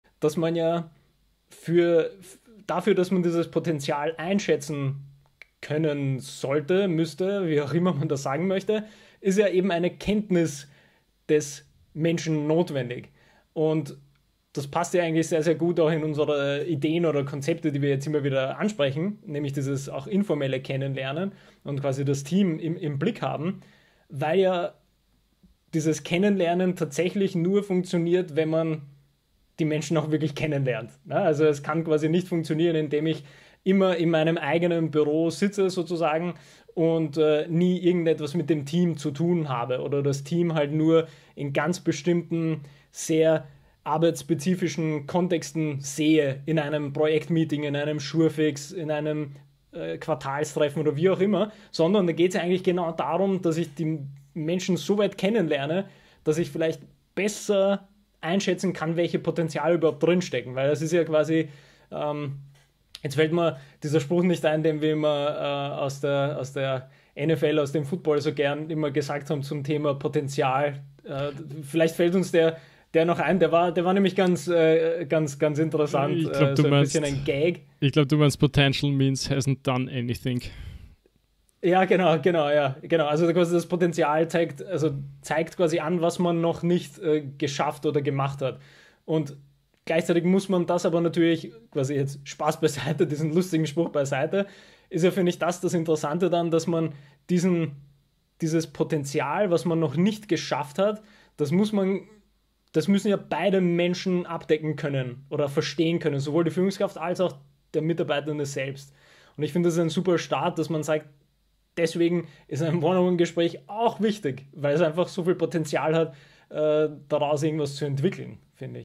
Wichtigkeit der informellen Beziehungsebene: Was kann ich als Führungskraft tun?
In diesem kurzen Clip spricht Bence über die informelle Beziehungsebene. Diese zu gestalten hat eine enorme Wichtigkeit und schafft die Basis für Vertrauen.